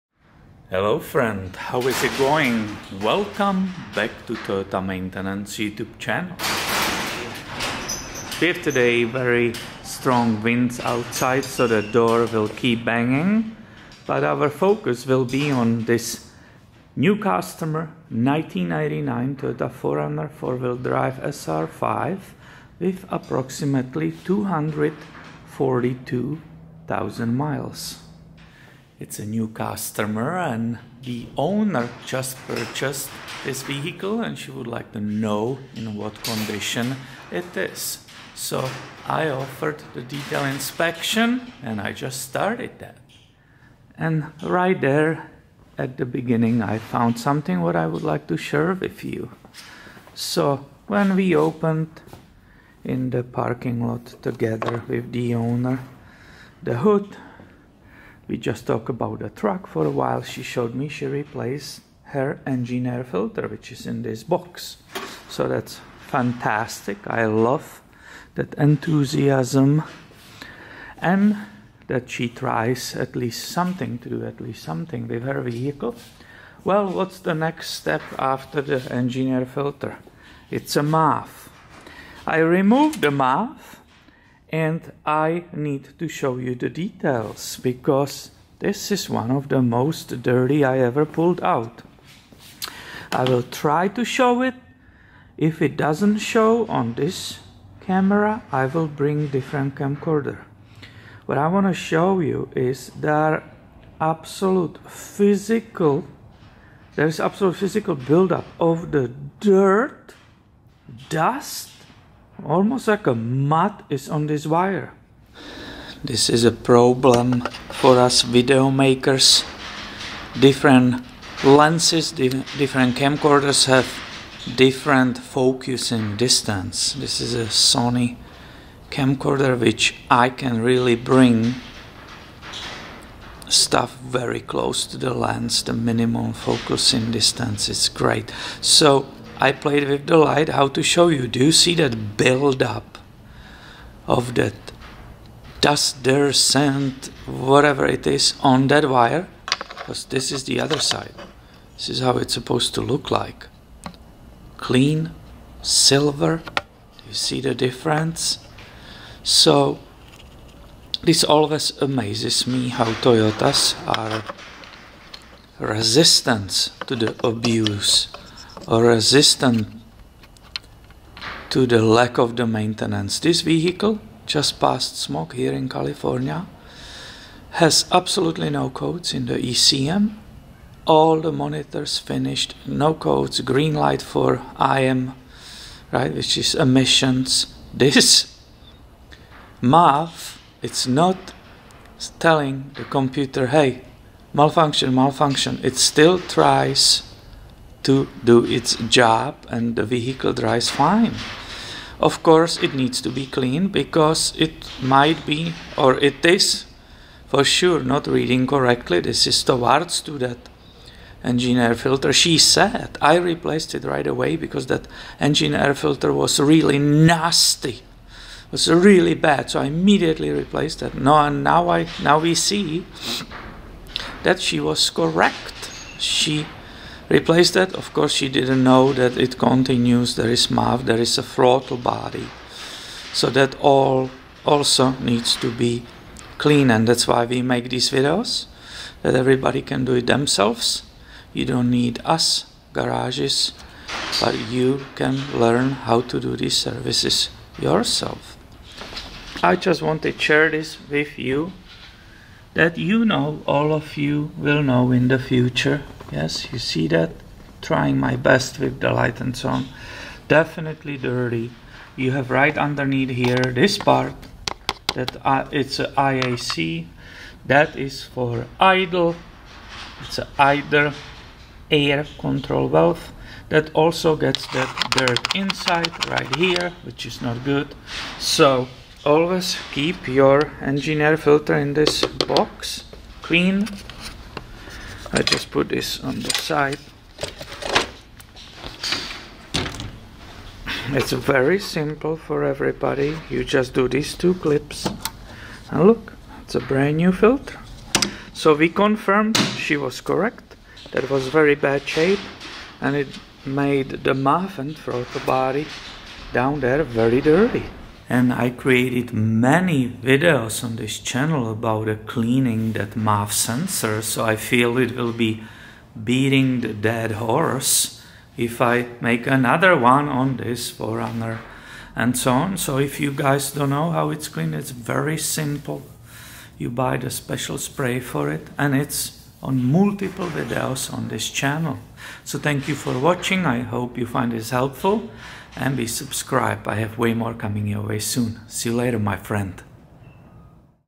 Dirty air filter - dirty MAF - dirty throttle body & IAC valve
If you don't replace engine air filter, you will get dirty MAF, throttle body and possible problems with IAC valve on this 1999 Toyota 4Runner